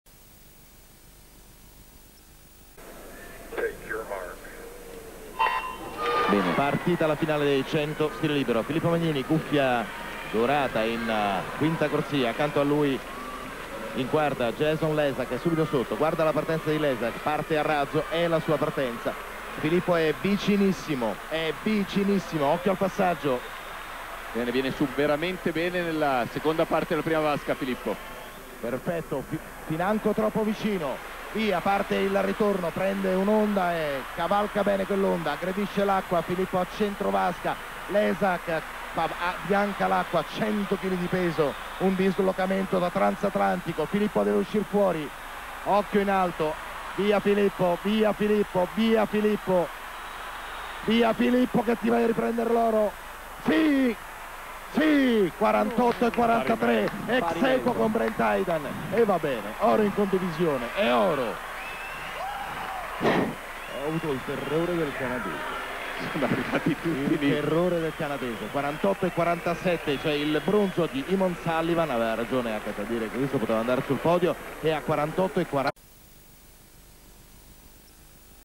NUOTO MONDIALI MELBOURNE 2007 L’ORO DI MAGNINI NEI 100 SL CON HAYDEN
ARCHEOLOGIA DELLO SPORT E’ SU FACEBOOK
25 LUG. 2017 In questi giorni in cui ricorrono i mondiali delle piscine, Archeologiadellosport ricorda quella mattinata nottata (italiana) di marzo a Melbourne in cui Filippo Magnini si conferma campione del mondo nei 100 stile libero, stavolta ex-aequo con il canadese Hayden.